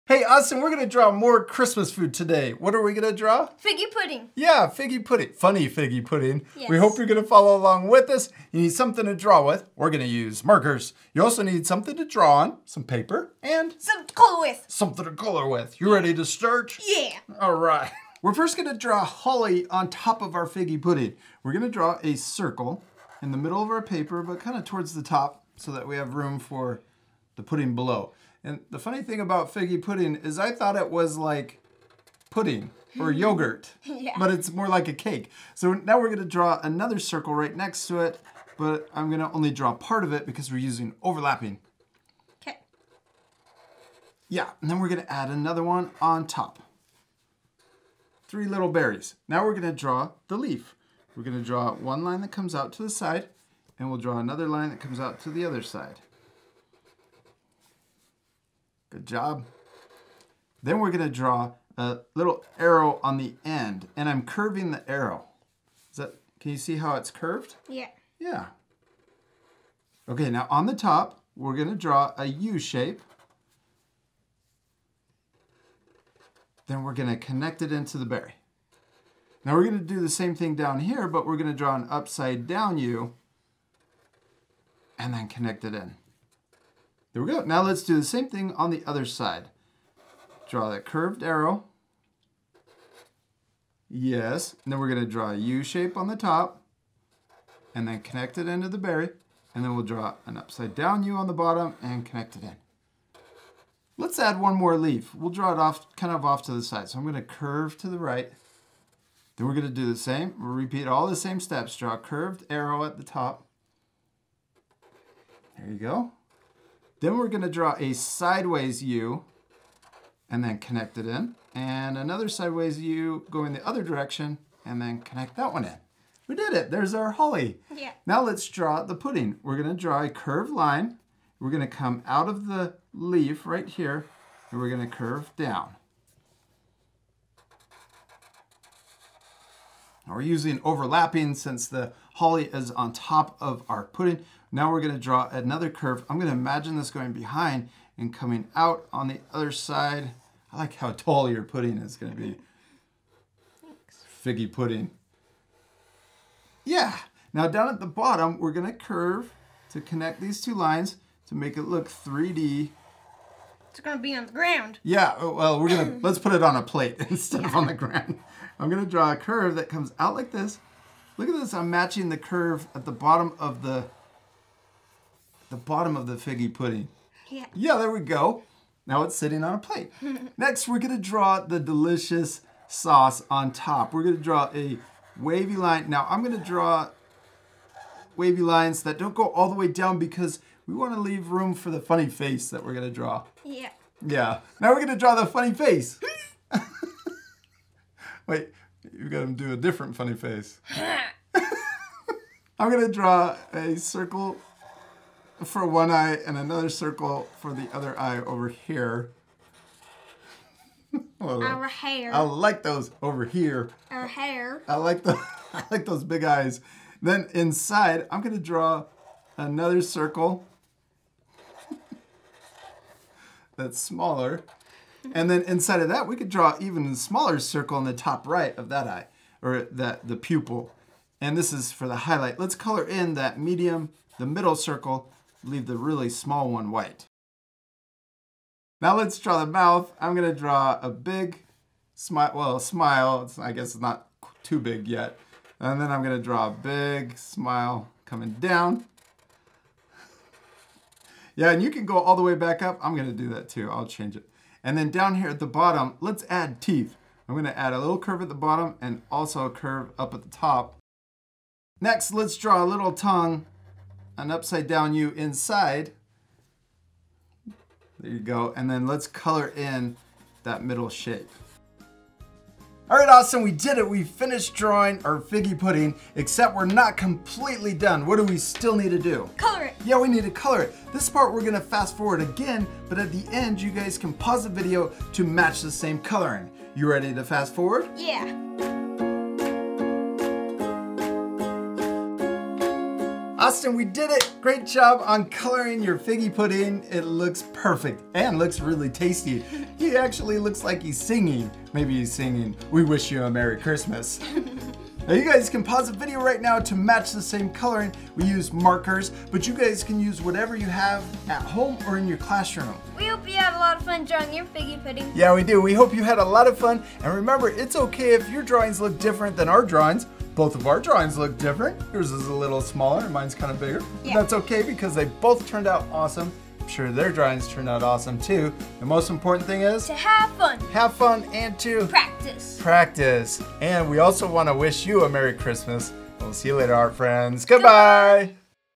How To Draw Funny Figgy Pudding For Christmas
Follow along with us and learn how to draw funny figgy pudding for Christmas!